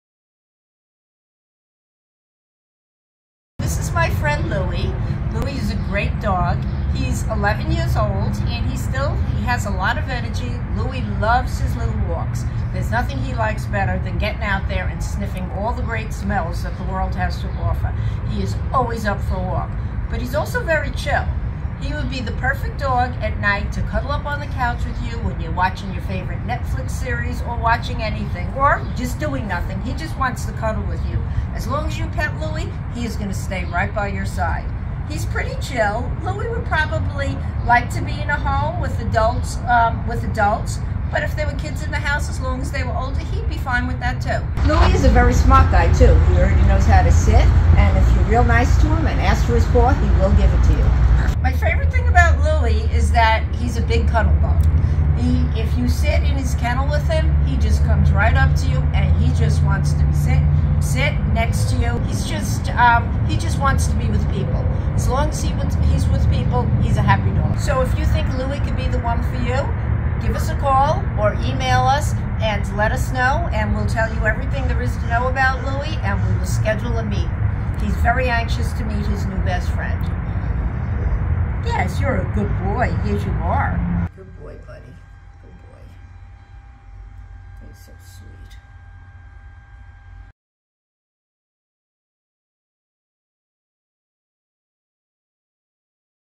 Meet Louie
Louie is as handsome as he is charming, and he's looking for a family to call his own.  Could it be you?  Learn more about sweet Louie now.